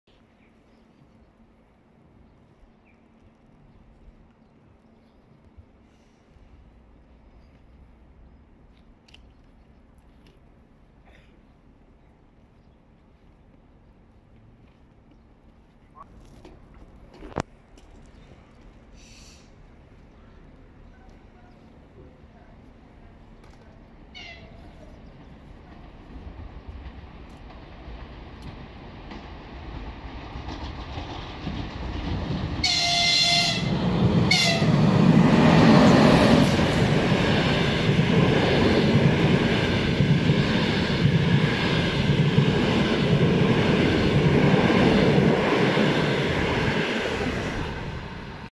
Double-decker train top view
Double-deck passenger train with two-system electric locomotive EP20-033 at Golutvin station, Kolomna, Russian Railways. View from the pedestrian bridge at the station.